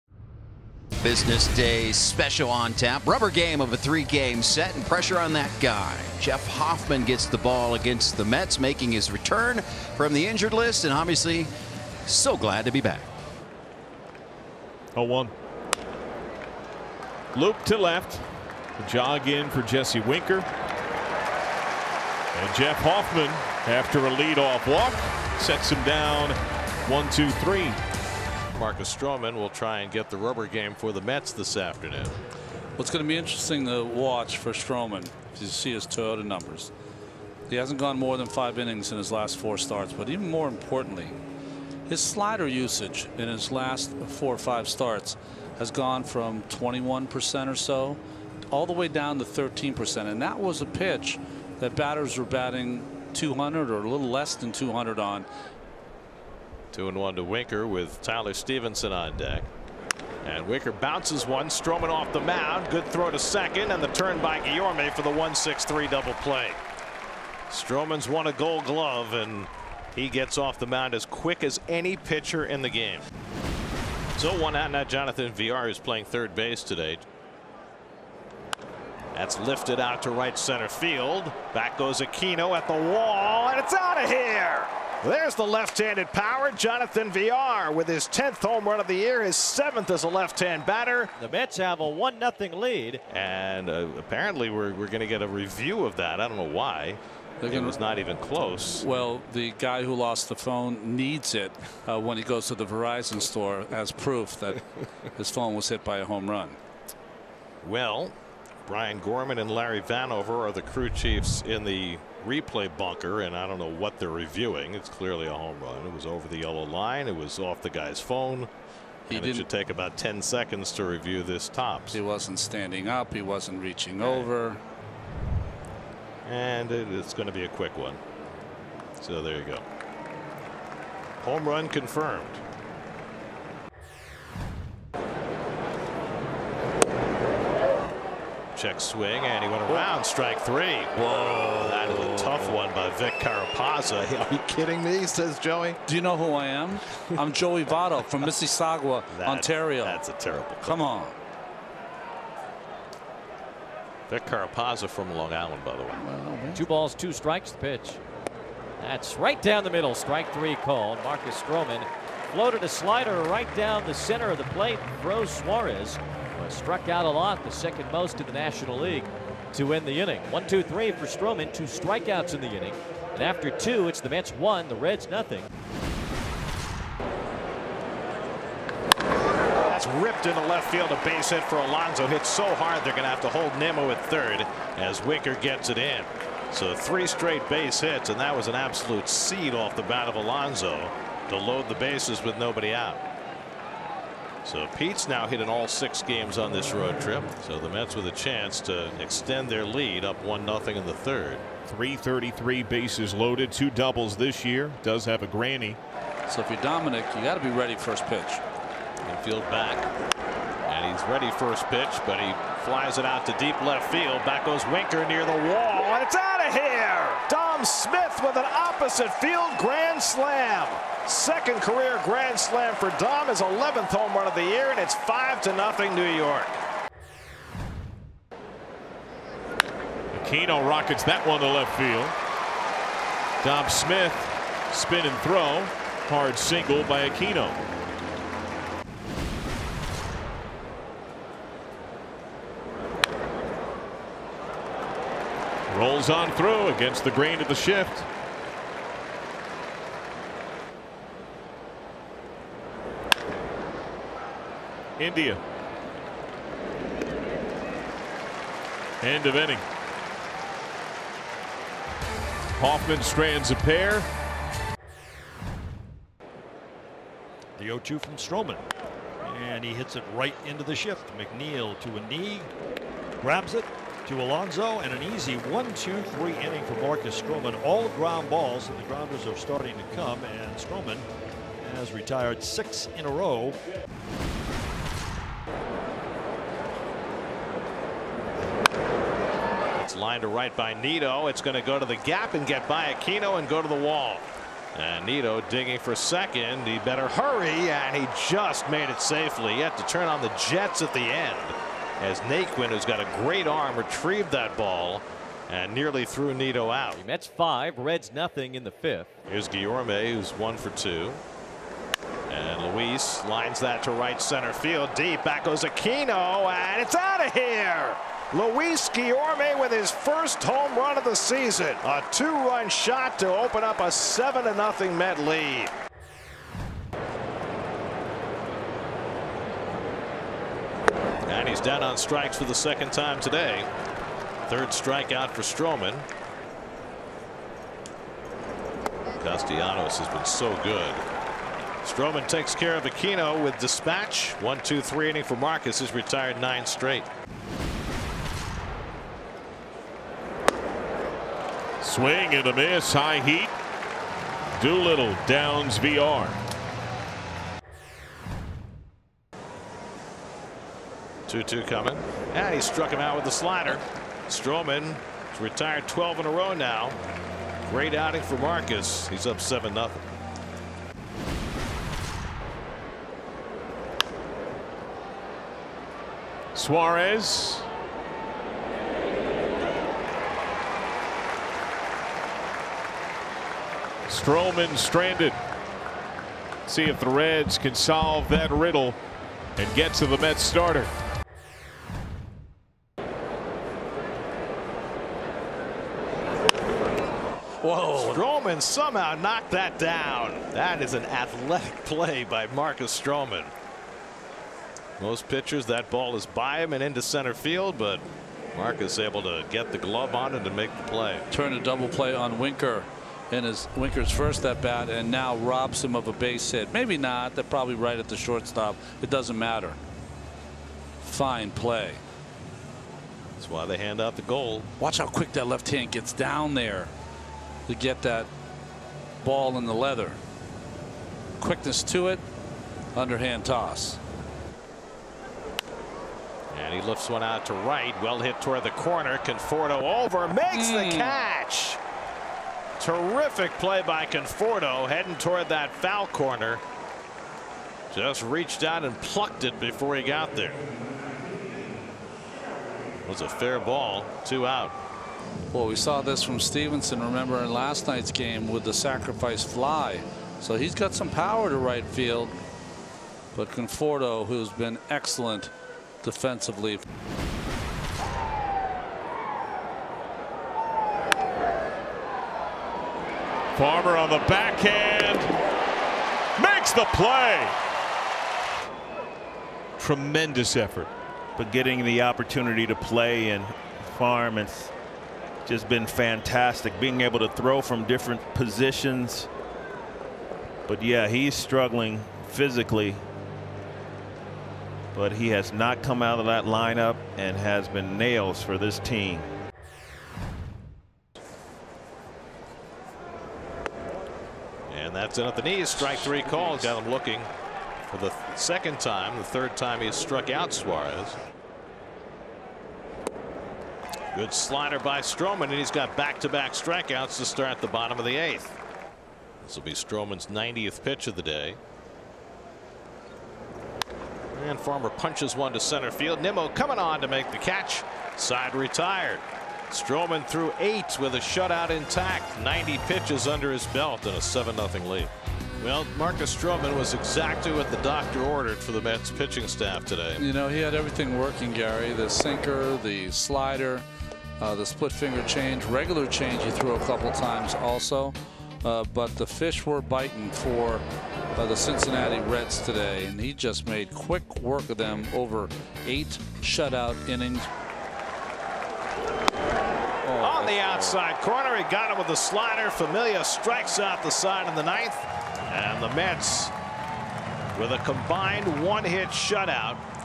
Stro Show Shows Off
7/21/21: Marcus Stroman absolutely dominated the Cincinnati Reds throwing 8 innings of shutout ball, allowing just one hit all day. The Mets bats exploded for three home runs, including a grand slam by Dom Smith.

Category
Sports